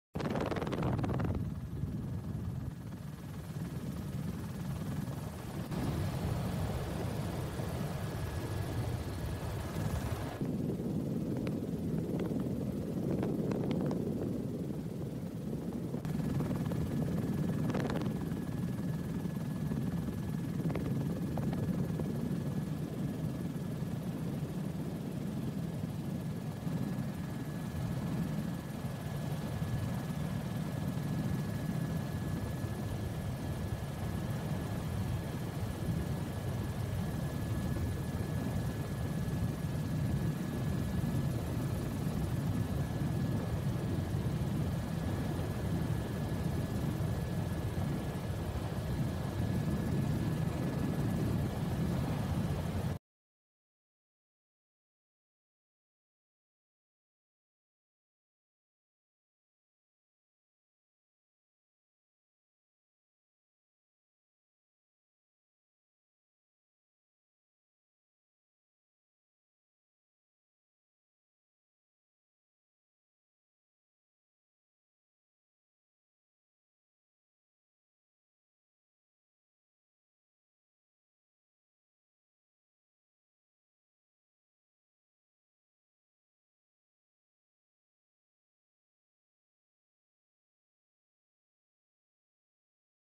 Aerial footage shows wildfires raging in Victoria Australia
Aerial footage shows wildfires raging in the state of Victoria Australia.